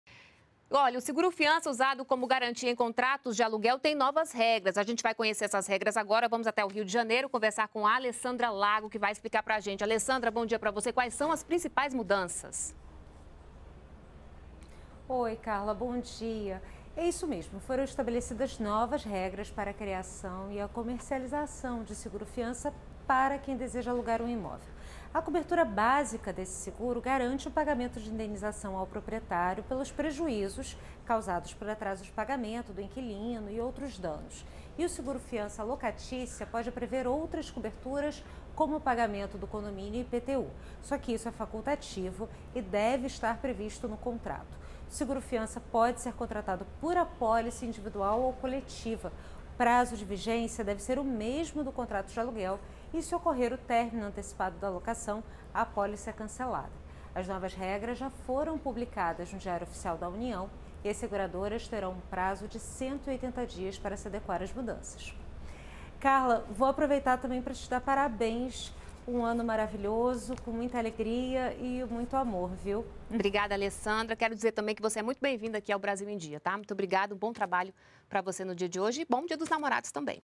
Seguro Fiança tem novas regras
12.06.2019 - O seguro fiança, usado como  garantia em contratos de aluguel, tem novas regras.